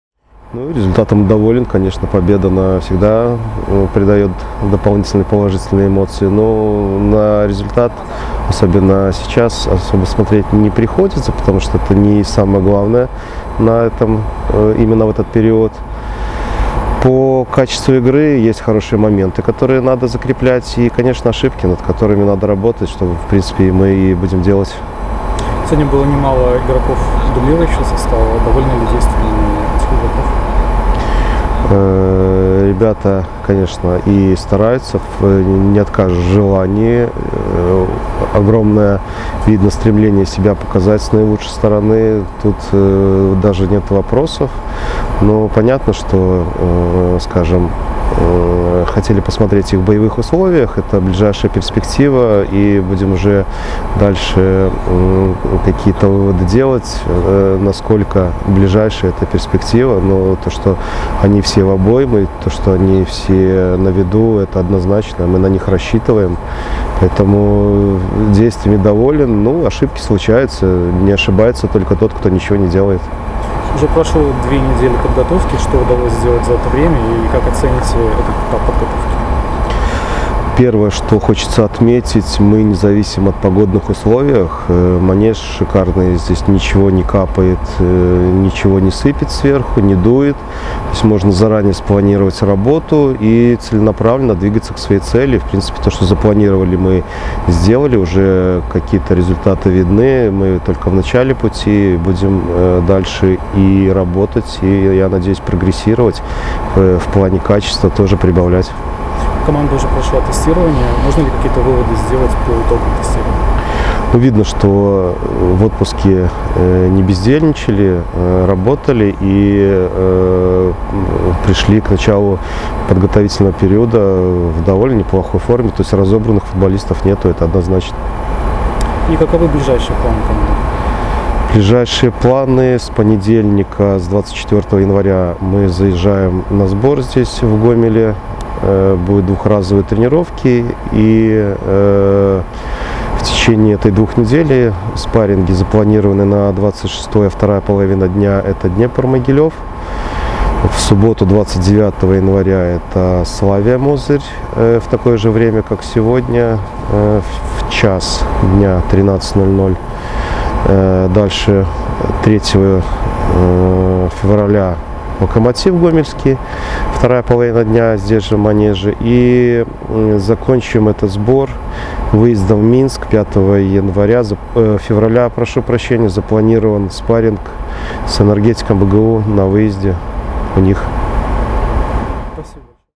Владимир НЕВИНСКИЙ | Комментарий после матча с "Белшиной"
Главный тренер нашей команды поделился впечатлениями по итогам товарищеской игры с бобруйской "Белшиной"